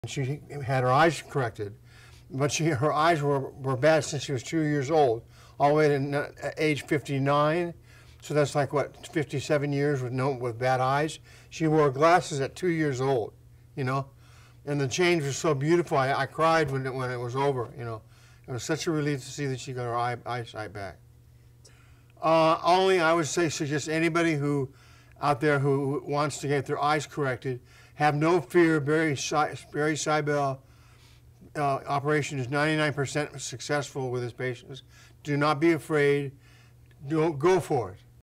Brian Wilson Beach Boys cataract surgery Dr. Barry Seibel Los Angeles eye surgeon
Brian Wilson of the Beach Boys comments on his wife's cataract surgery performed by Dr. Barry Seibel, Los Angeles eye surgeon LASIK